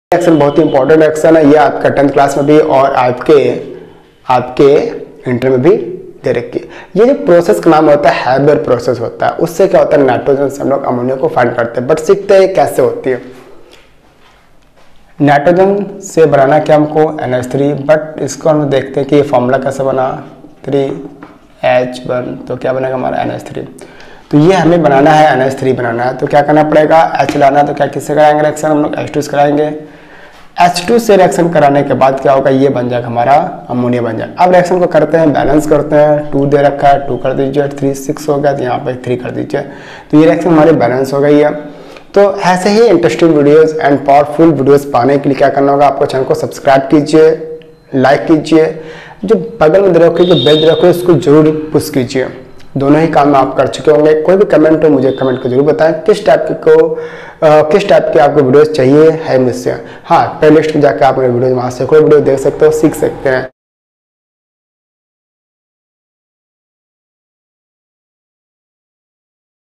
Nitrogen to Ammonia| nitrogen se ammonia|N2 to NH3| inorganic chemistry reaction
I think aap ko video accha laga hoga
Aur Ghanta 🔔bajana mat bhoolna .for upcoming videos updates.